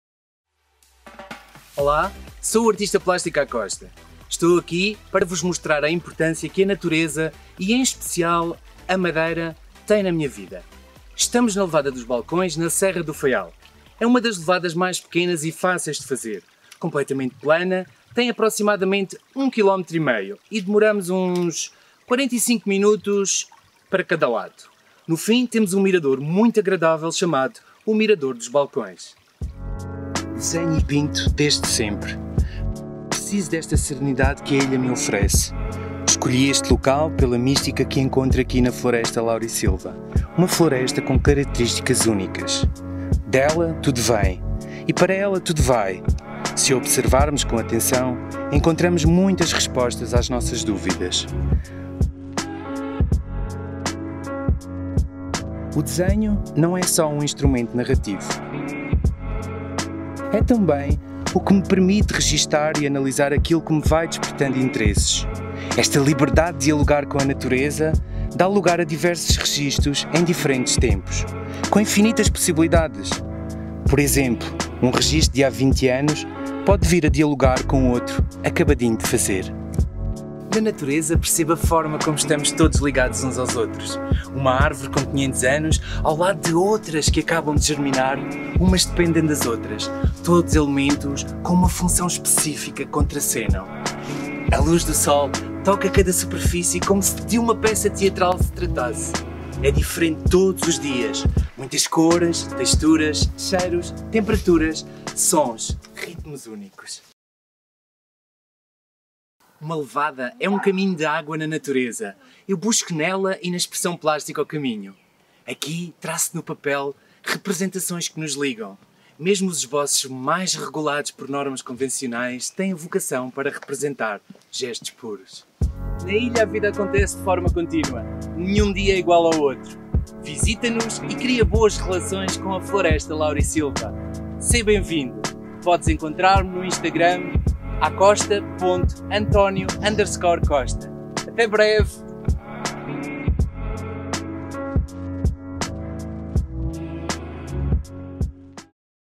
LEVADA WALKS IN MADEIRA WITH ARTIST ACOSTA!
Exploring the stunning Laurrisilva forest of madeira with @acosta.antonio_costa as he explains what Madeira means to him and how nature has influenced & guided his artwork.

more content on instagram @exploringMadeira